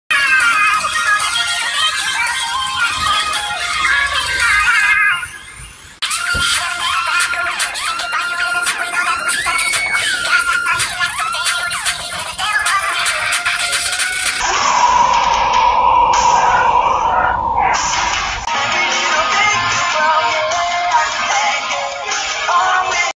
What Boredom Does
Lalalala.

I got out of class early. Had a camera, found the effects area, and decided to recorded myself.